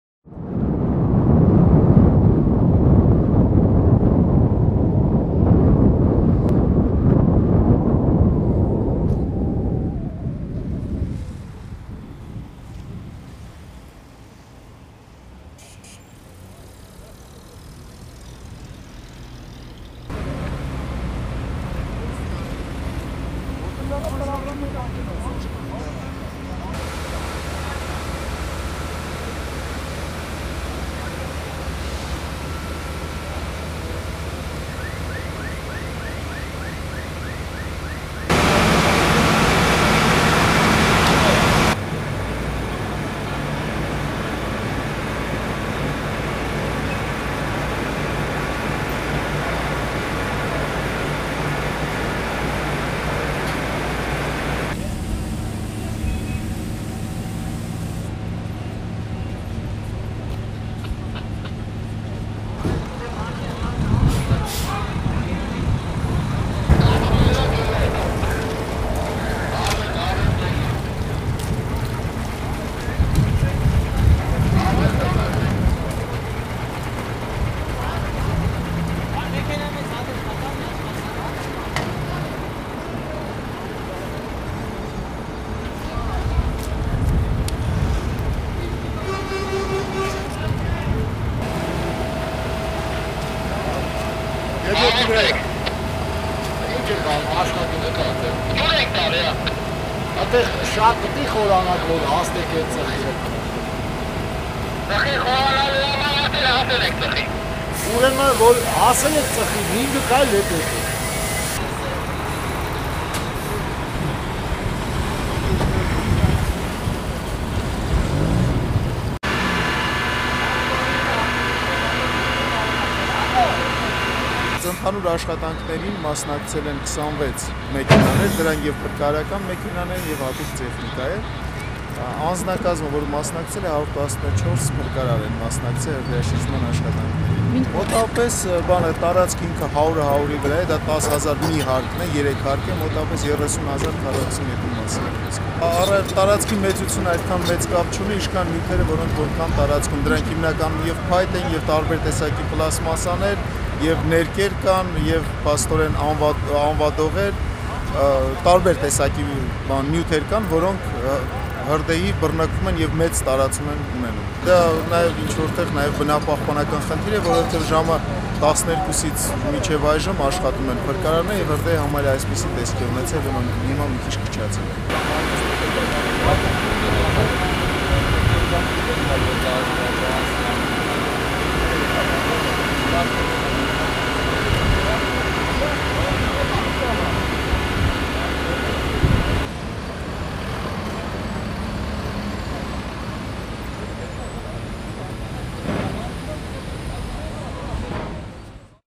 Երևանում 30 հազար ք/մ տարածքն այրվում է ավելի քան 12 ժամ
ԱԻՆ բնակչության պաշտպանության և աղետների հետևանքների վերացման կազմակերպման վարչության պետ Հովհաննես Եմիշյանը տեղեկացրեց, որ Սպայկա ընկերությանը պատկանող այս շենքը լի է դյուրավառ նյութերով, ինչի պատճառով հրշեջների մարած վայրերում կրակը նորից է բռնկվում | HotTV լուրեր